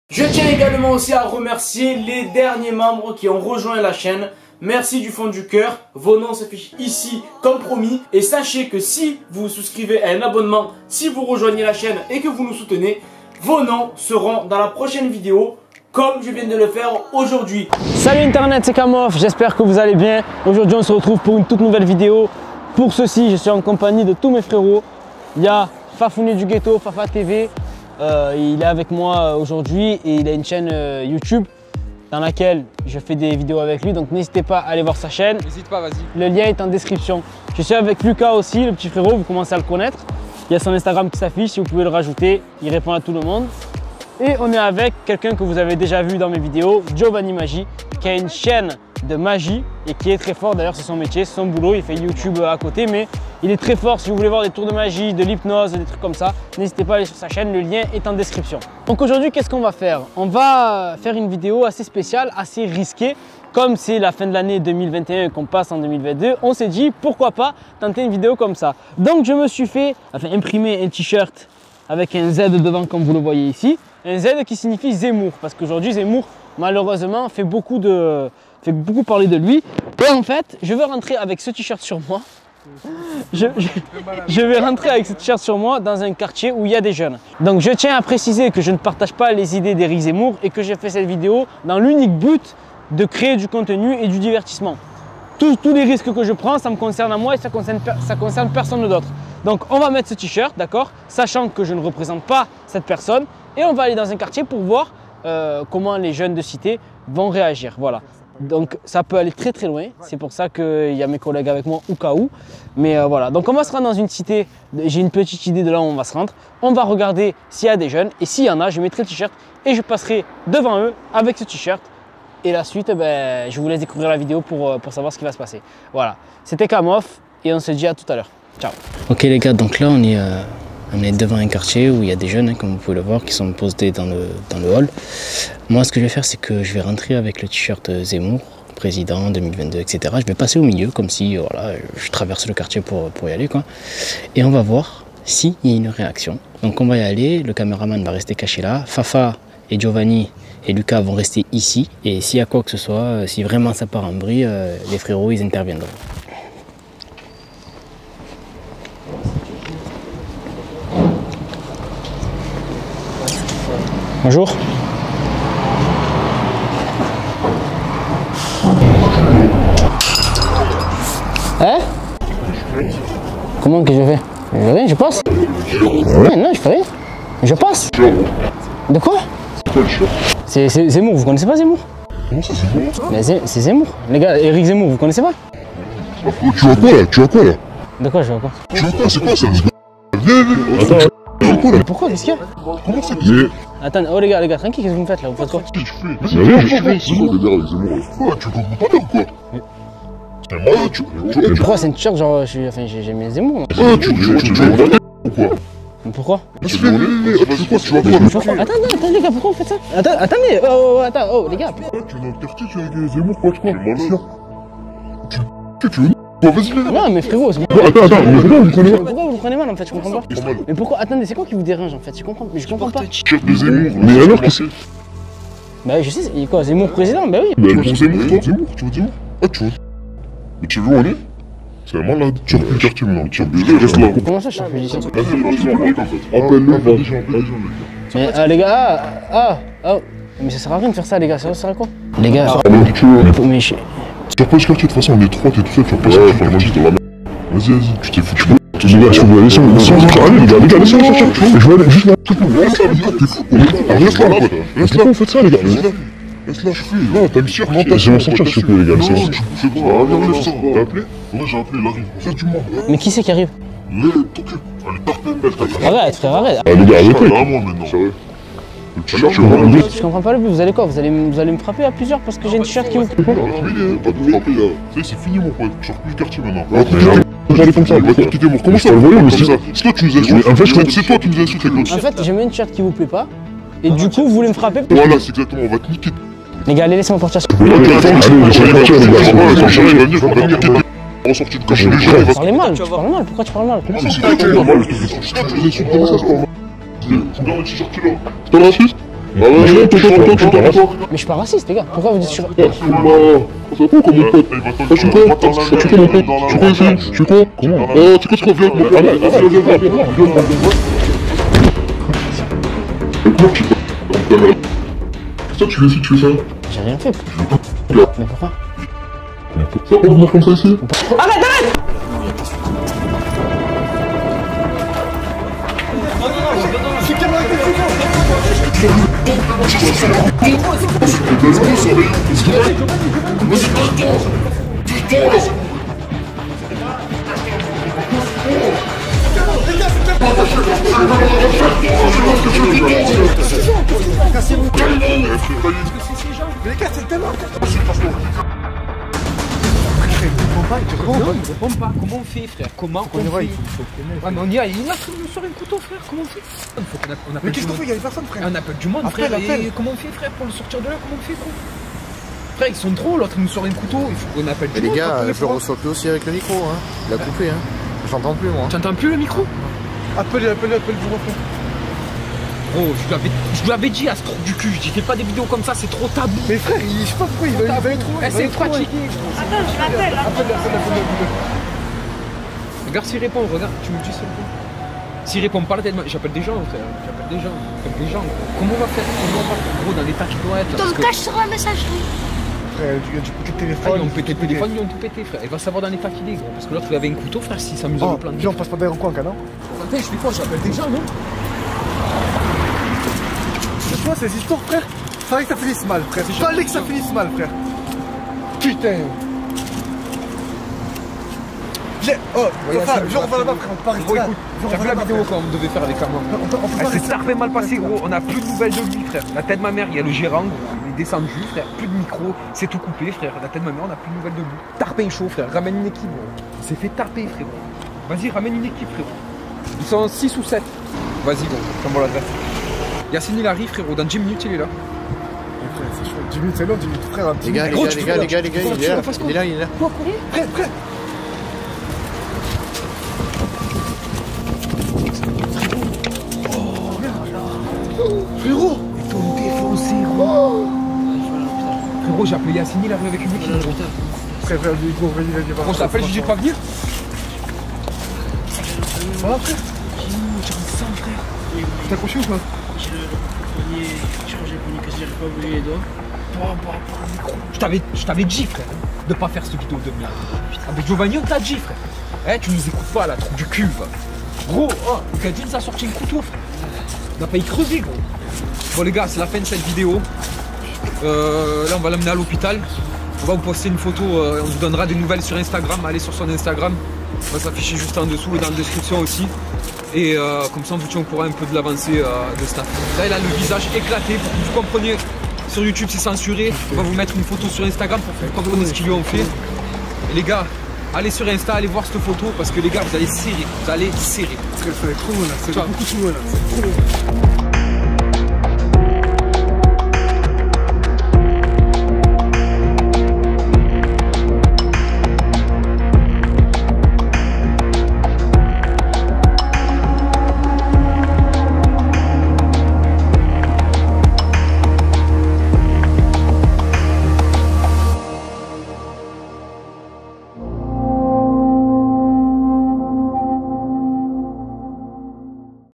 JE PORTE LE T SHIRT DE ERIC ZEMMOUR DANS UNE CITÉ CHAUDE ! CAM OFF
Et LIKE 👍 pour le référencement ! Merci d'avance !